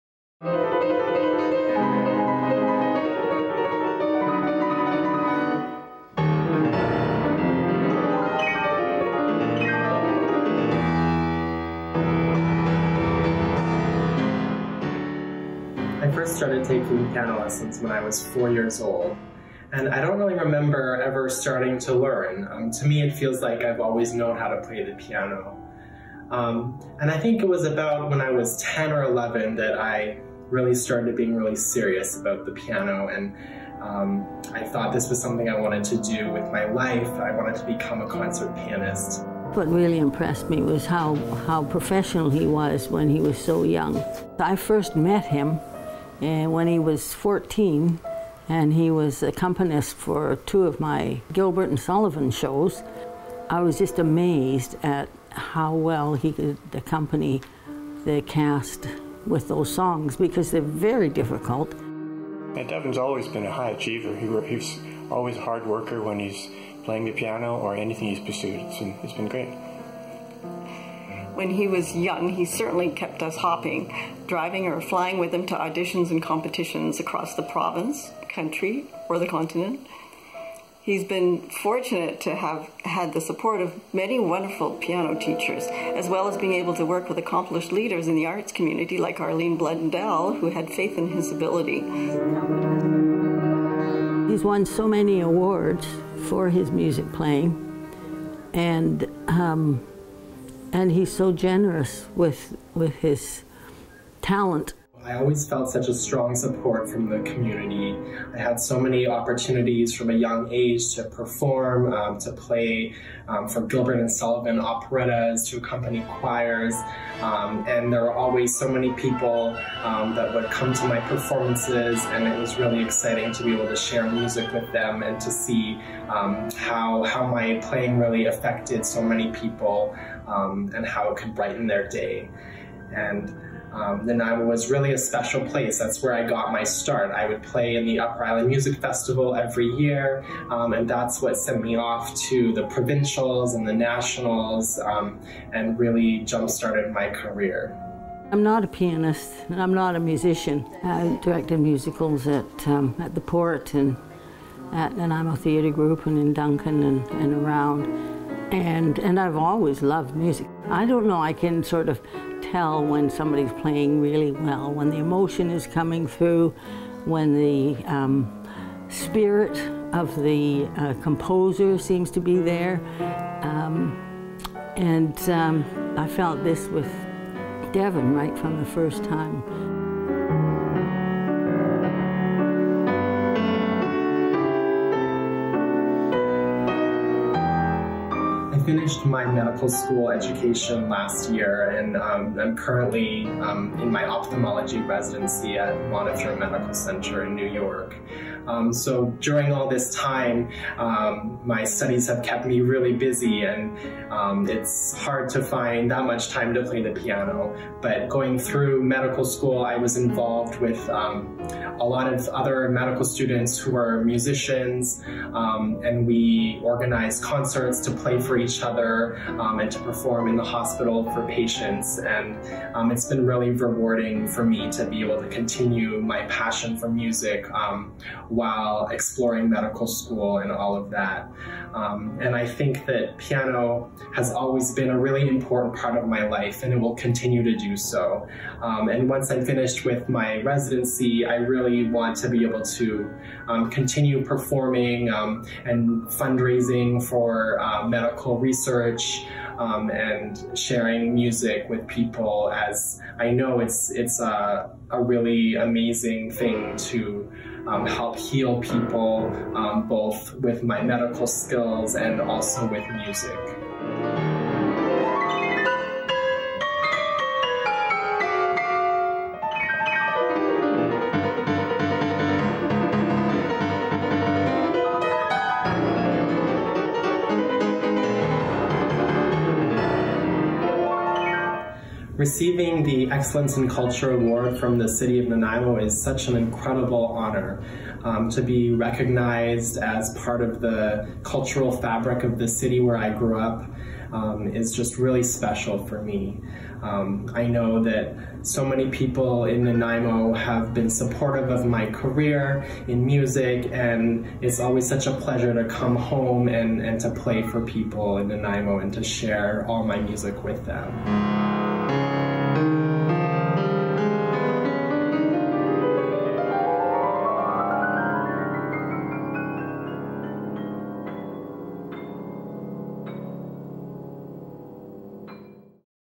Devon Joiner, Excellence in Culture 2020 (CITY of NANAIMO)
Devon Joiner is named as the 2020 Excellence in Culture Award recipient for his international award-winning career as a concert pianist. Devon has made numerous guest appearances with the Nanaimo Youth Choir, the Camerata Singers, the Island Bel Canto Singers and the Vancouver Symphony Orchestra. He has performed across Canada and the US, as well as in Asia, Europe and Africa, making his orchestral debut with the Vancouver Symphony Orchestra (2007) and Carnegie Hall (2011). In addition to his performing career, he has been the collaborative pianist for the Children’s Highbridge Voices choirs in the Bronx. Devon has taught many students, including five weeks of arts empowerment workshops in Tanzania with the Arusha Arts Initiative. Learn more about Devon and the City’s Culture & Heritage Awards

Videos by Kelly Adams Video & Photography